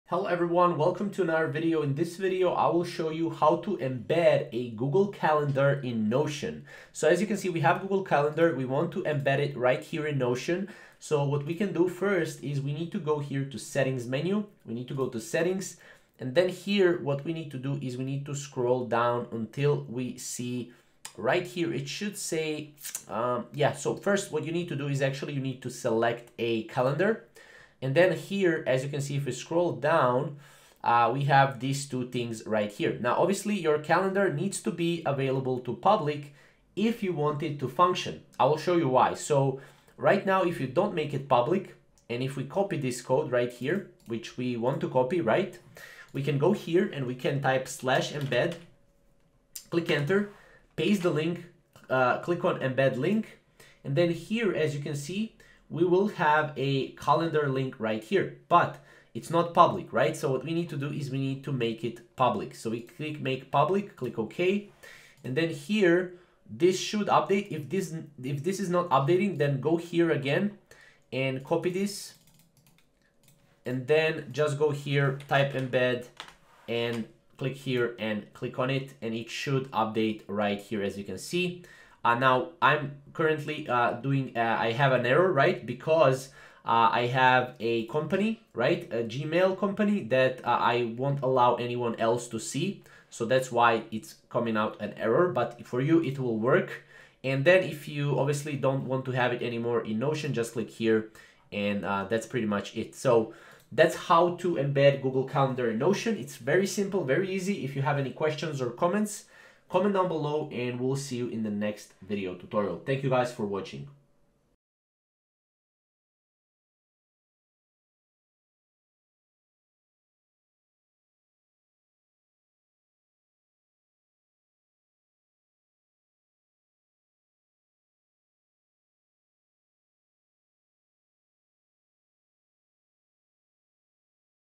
How to Embed Google Calendar in Notion (2024)
In this video, I will show you how to embed google calendar in notion